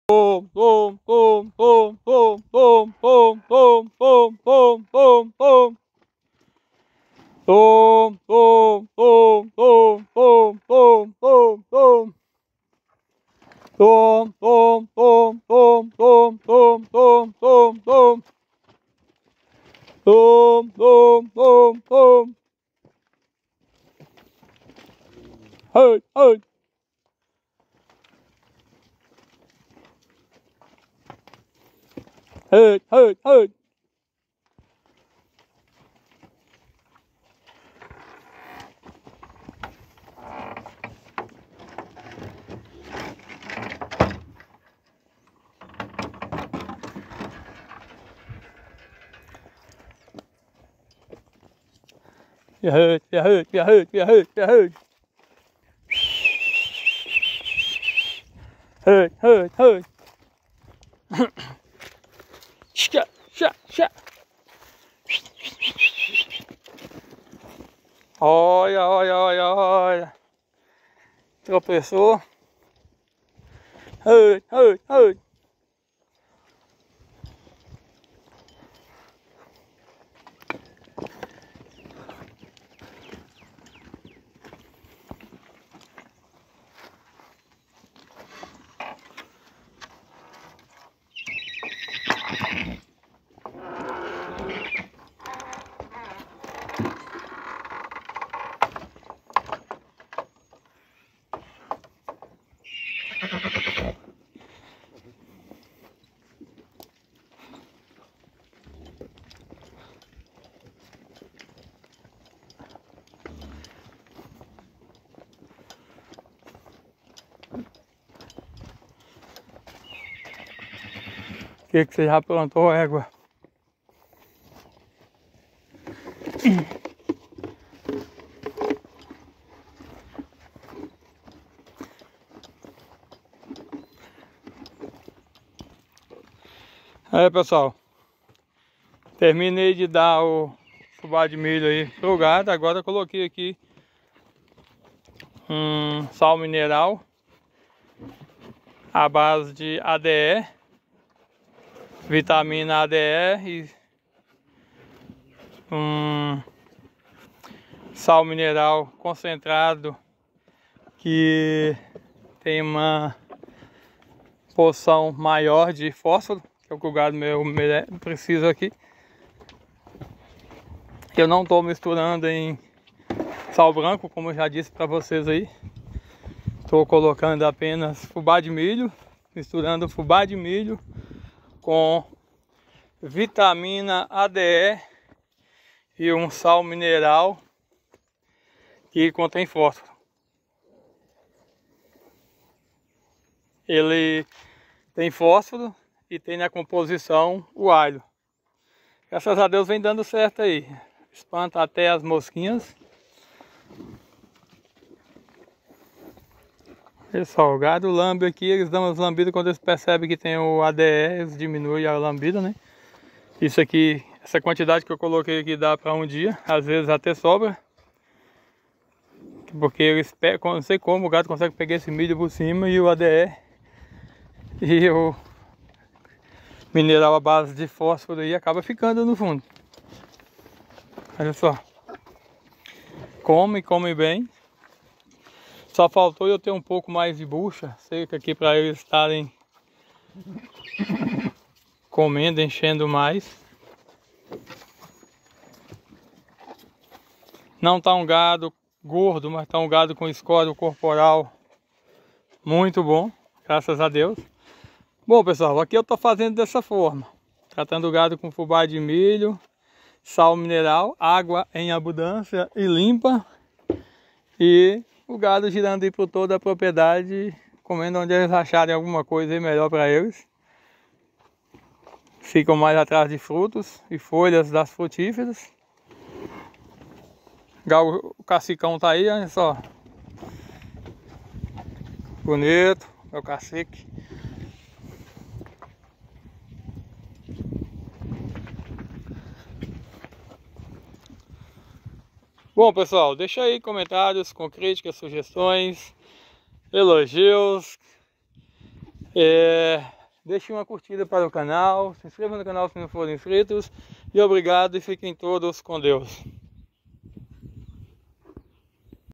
VITAMINA A D E E SAL MINERAL MISTURADO AO MILHO 🌽 EXCELENTE PARA GADO NO PERÍODO DE ESTIAGEM
Sitio Sou da ROÇA